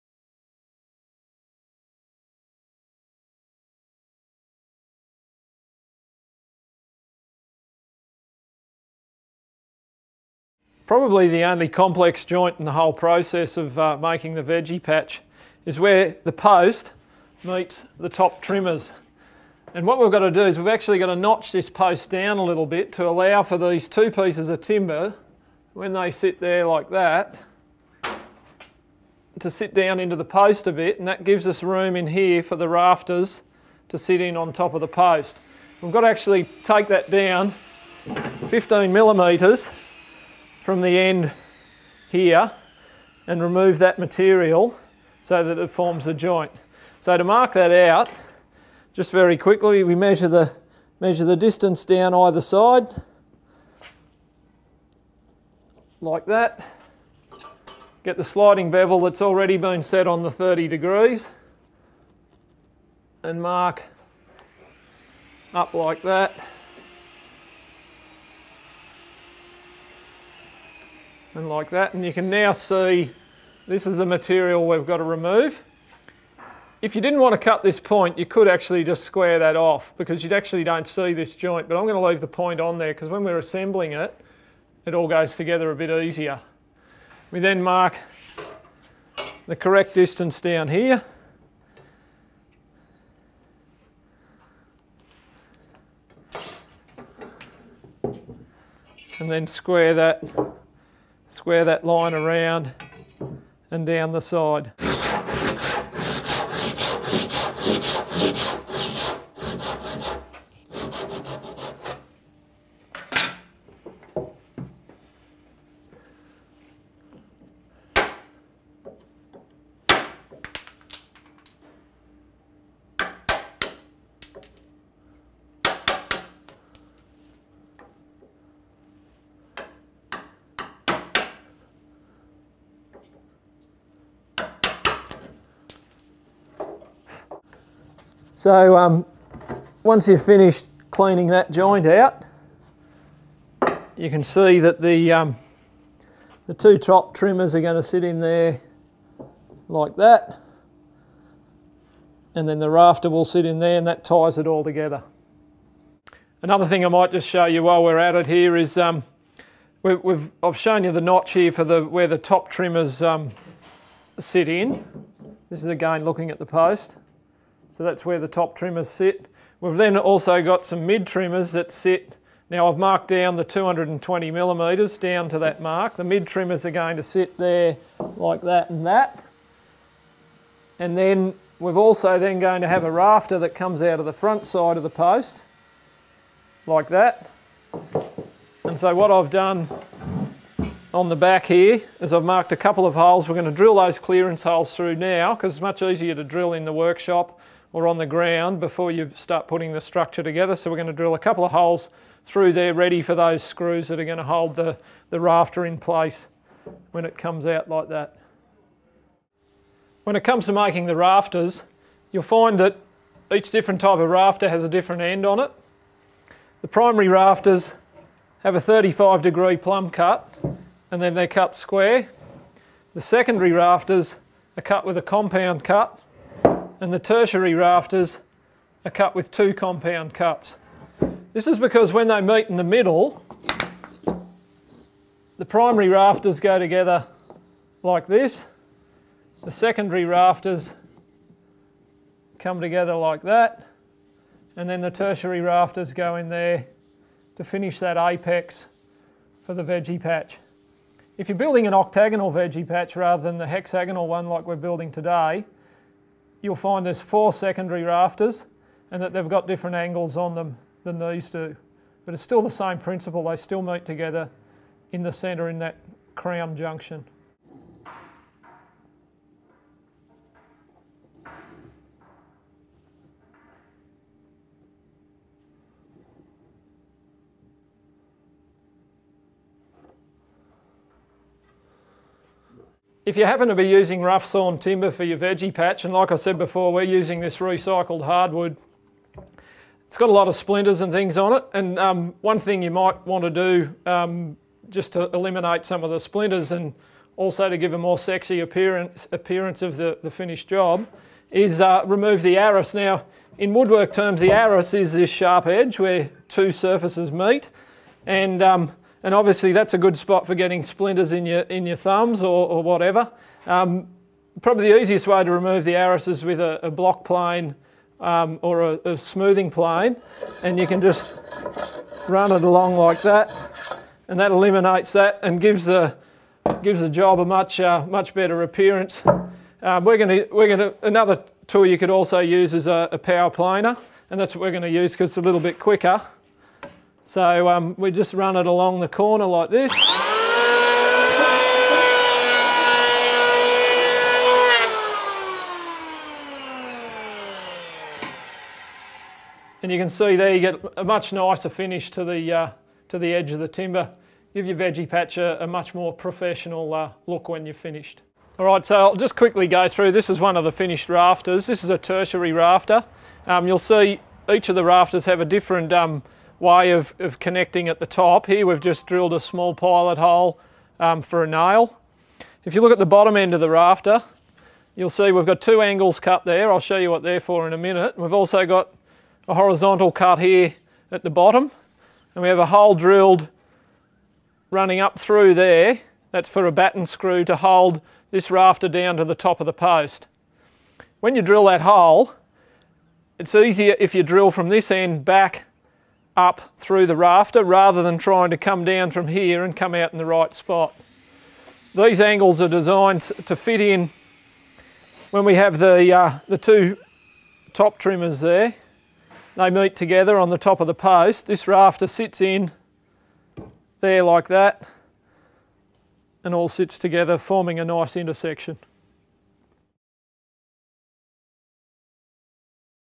2. Build a permaculture vegetable enclosure designed by Chris Francis.. Part 2 of 8
Grow your own organic vegetables. Learn how to prepare the timber components required to make this 6 bay permaculture vegetable enclosure including setting out and cutting the joints. This is part two of an eight part series that takes you through the entire construction process presented by Chris Francis of Strongbuild.